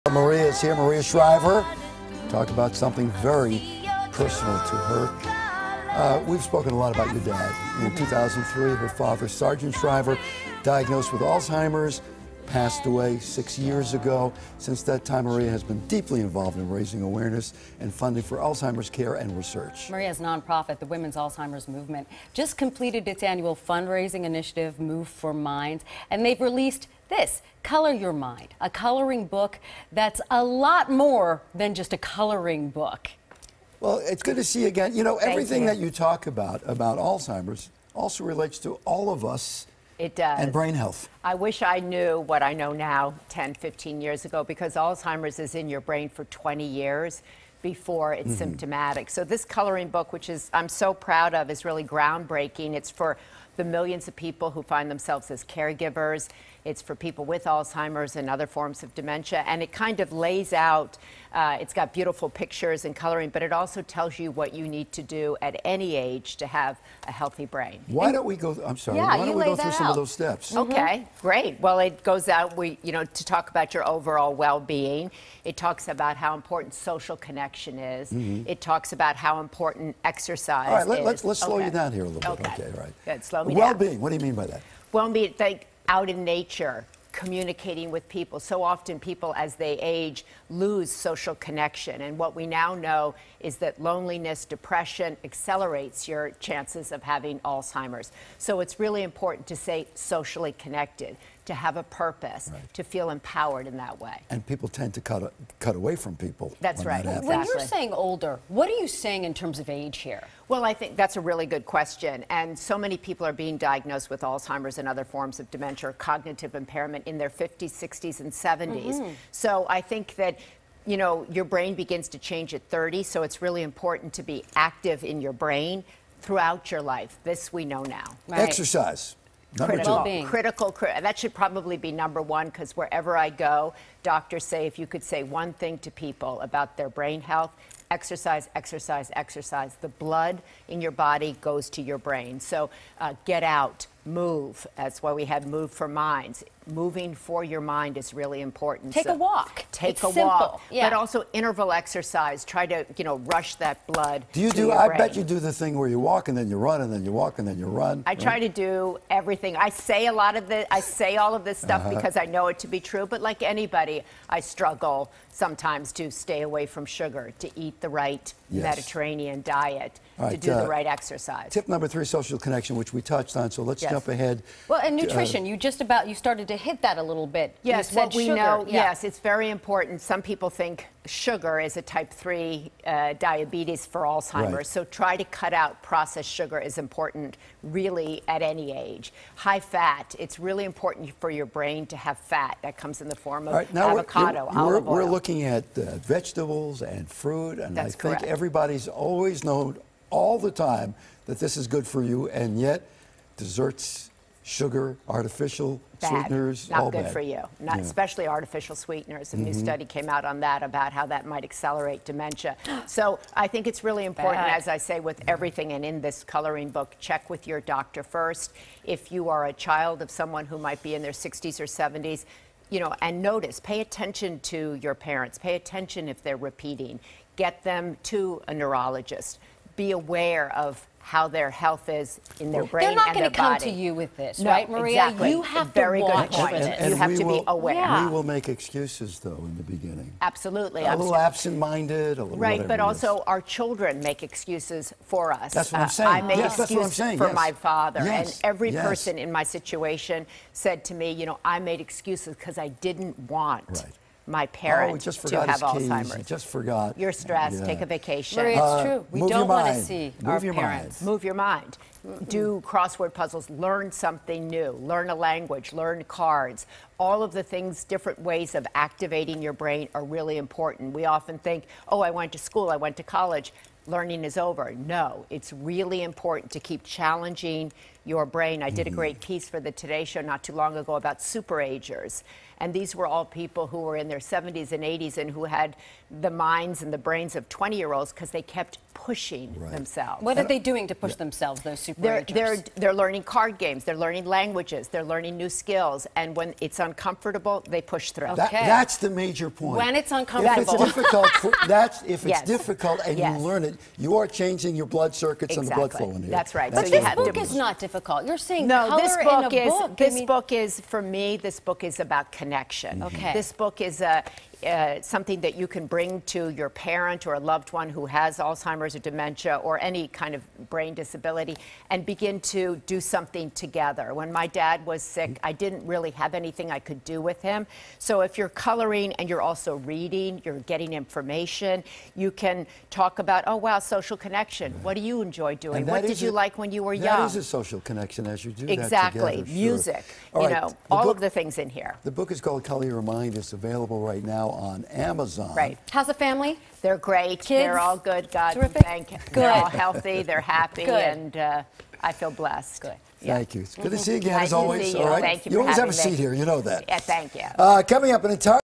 Maria Shriver continues fight against Alzheimer's with 'Color Your Mind' book
Maria Shriver developed a coloring book with insights from caregivers, neurologists, psychologists, and, of course, people with Alzheimer's. More